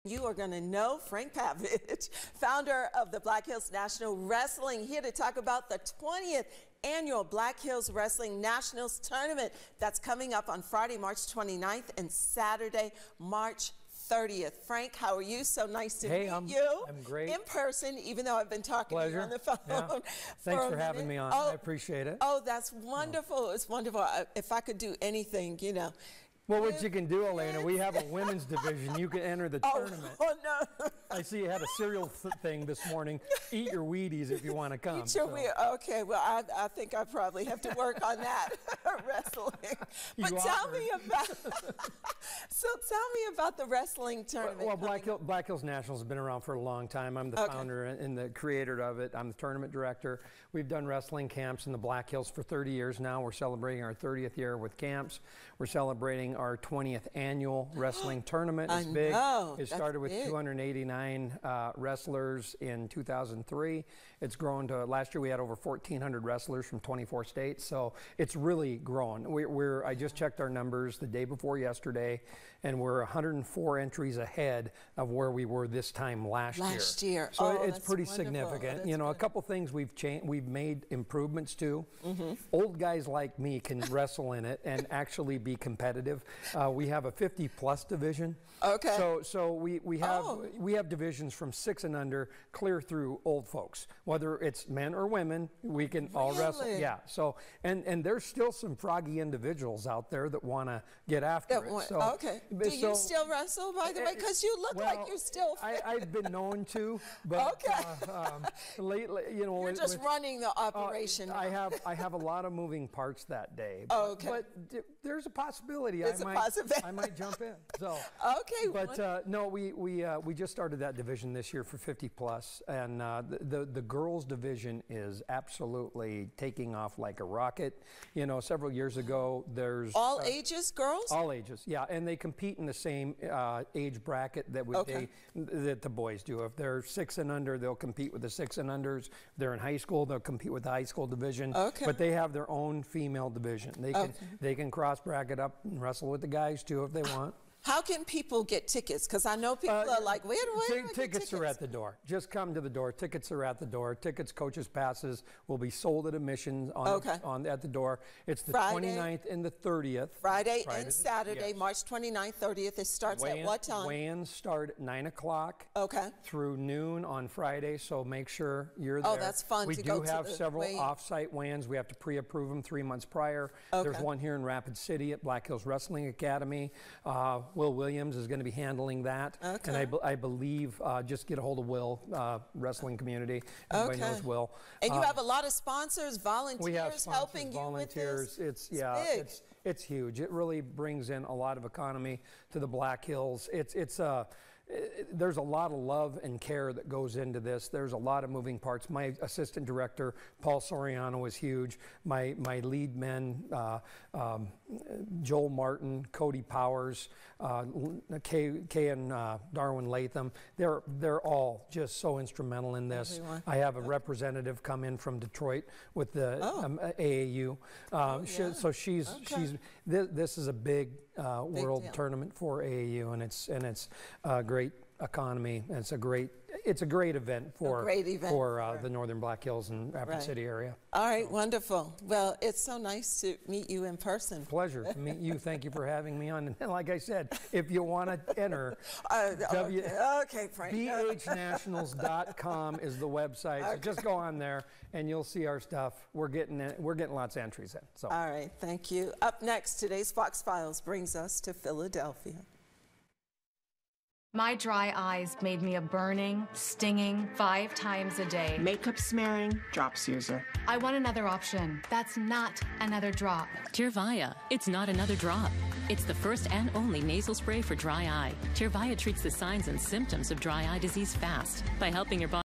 Its gonna be wrestlemania fun as popular Black Hills sporting event returns to Rapid City
Major wresting tournament prepares to take over the Black Hills